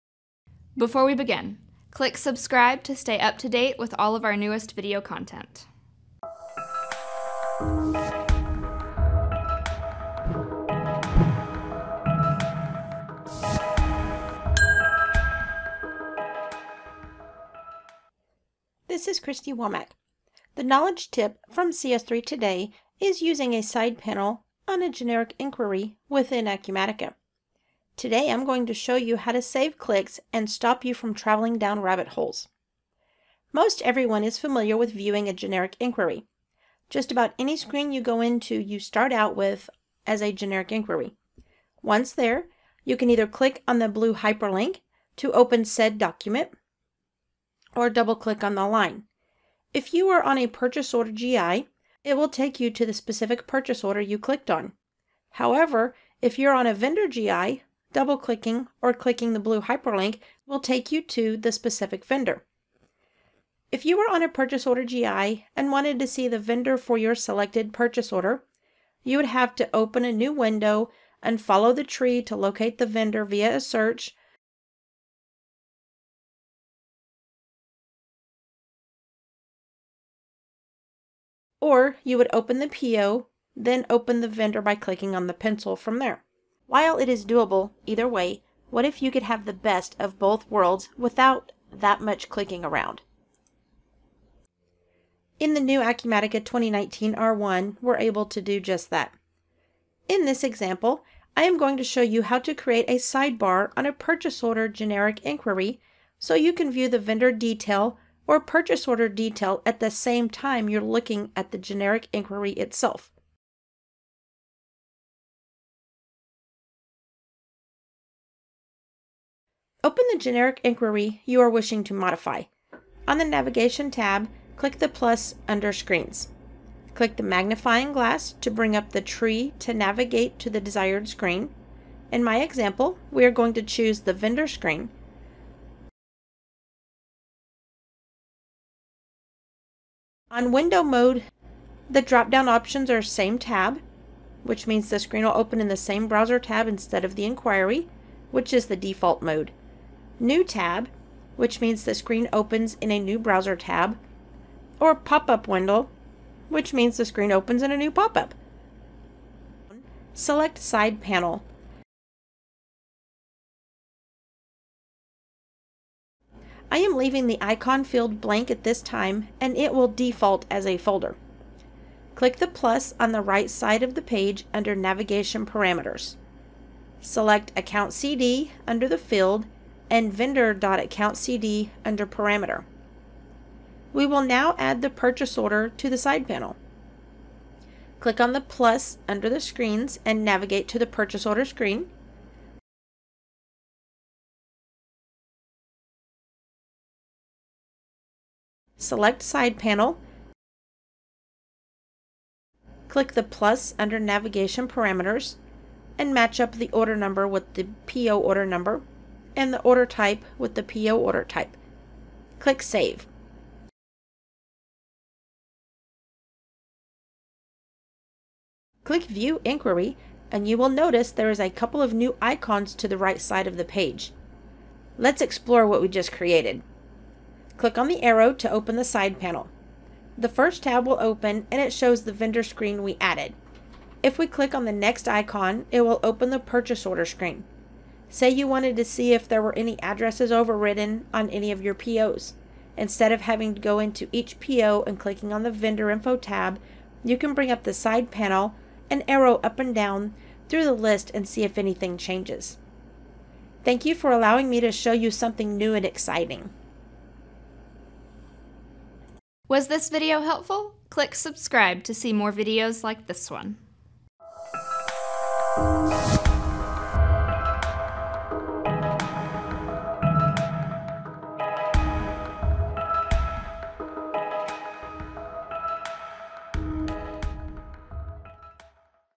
Acumatica Side Panels for Generic Inquiries
This video will show you how to add a side panel that shows detail information about each line on your generic inquiry in Acumatica. For instance, in a list of opportunities, you could show the business account record on the side. Or on a list of open accounts payables, you could show the vendor master record. Acumatica has done it again!